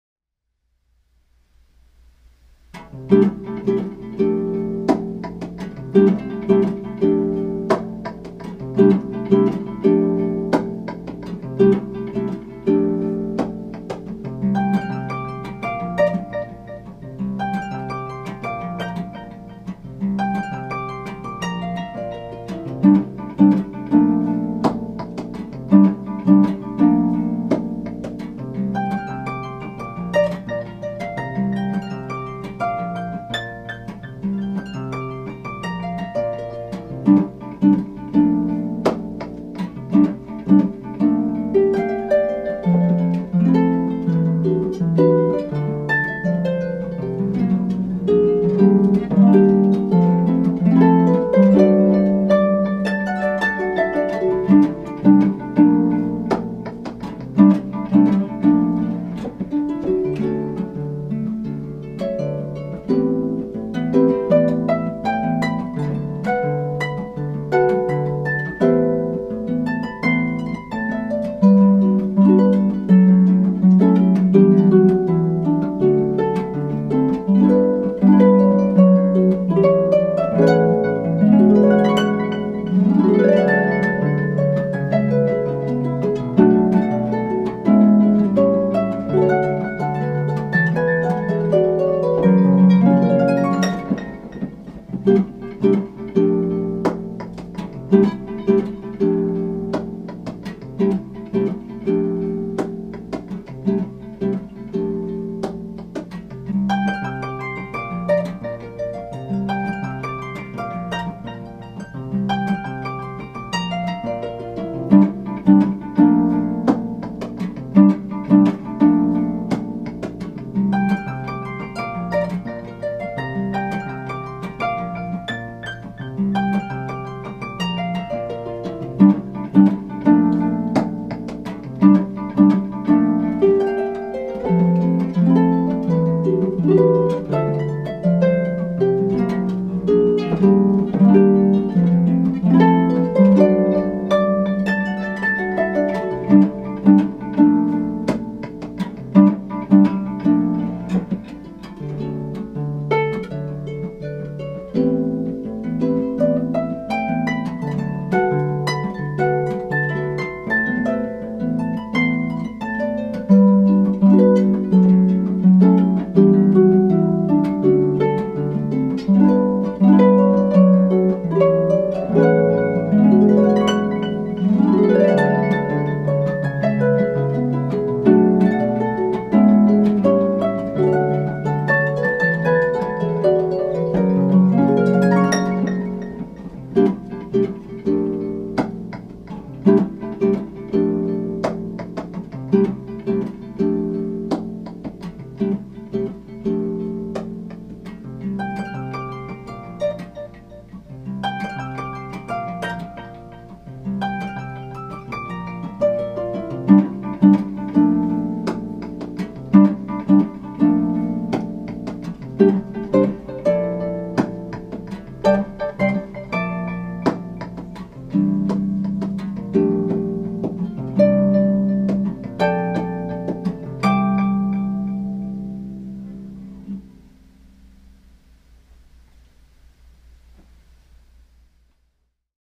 Suspended Harp camera (workshop) circle open, close
Suspended Harp camera (workshop) clean vid with circle open and close, camera on harp only